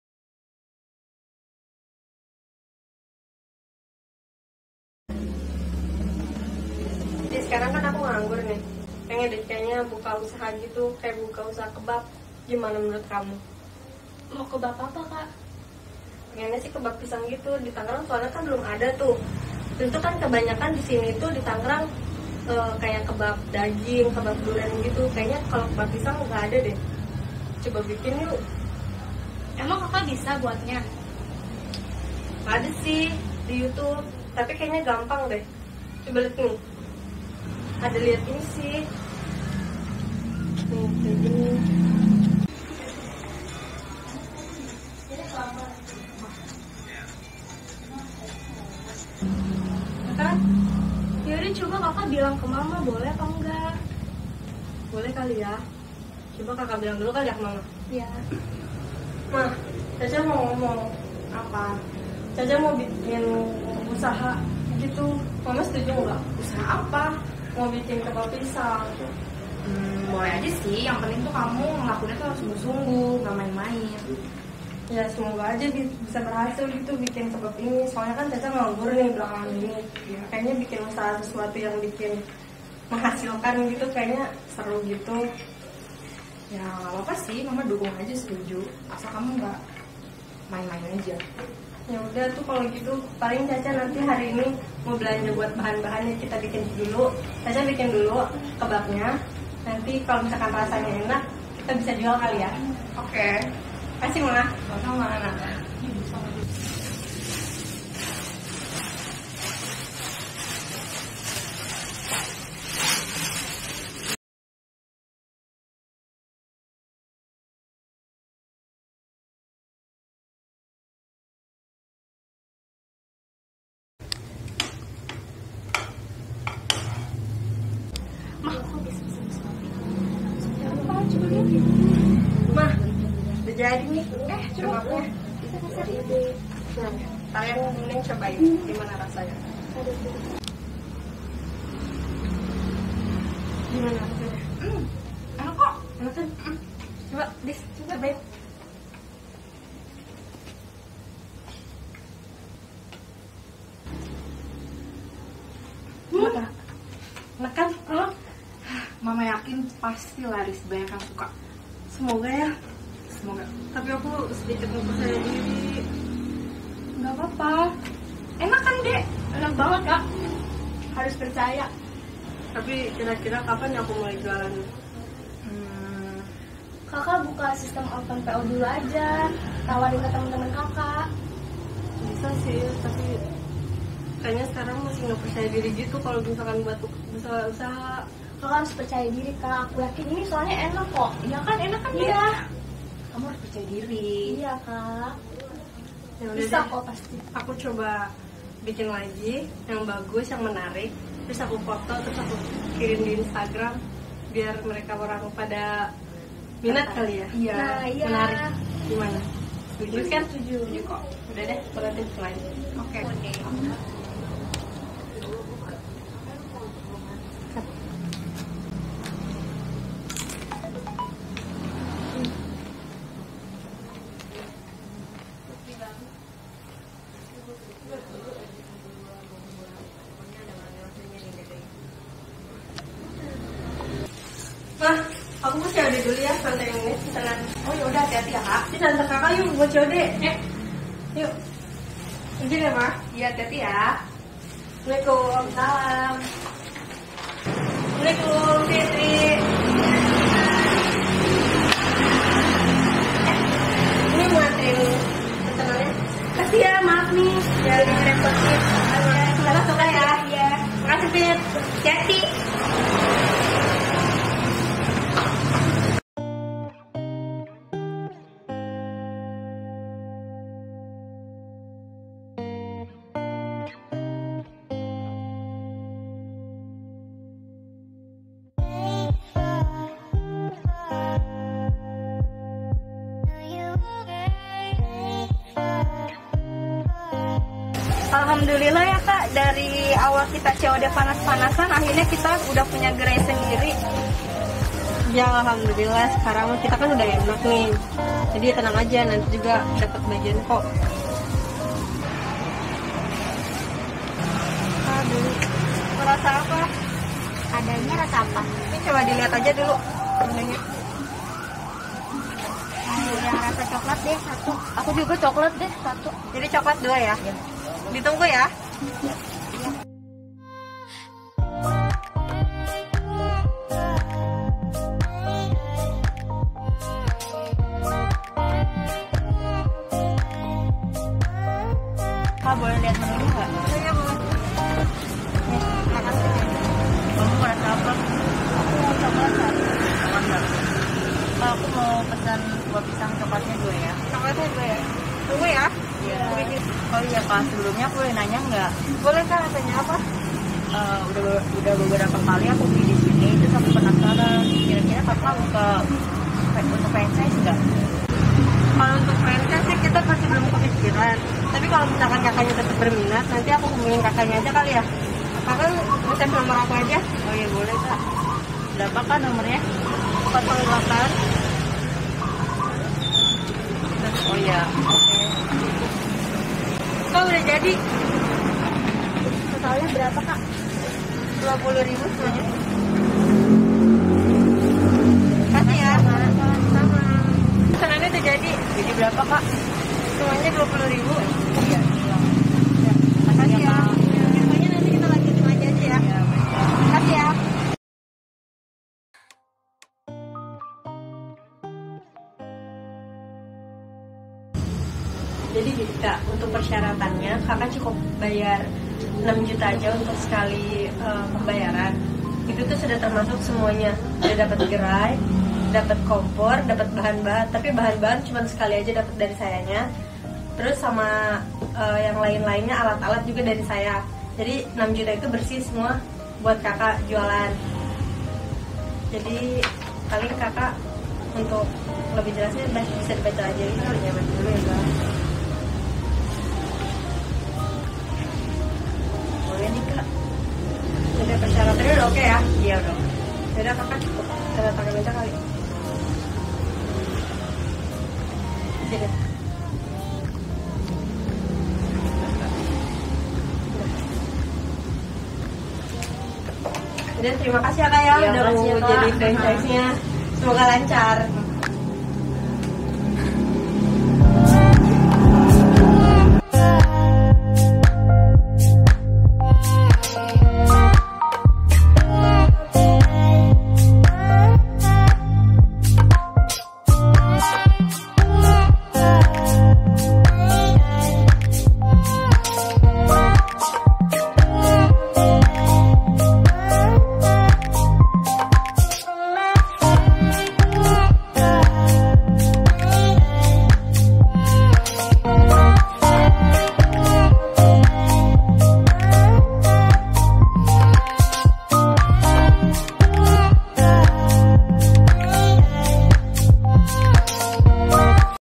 Kebab Unik Rasa Asik
Meski kondisi pandemi belum mereda, niatan untuk berbisnis tidak boleh padam.  Selalu ada jalan untuk menggali ide-ide bisnis.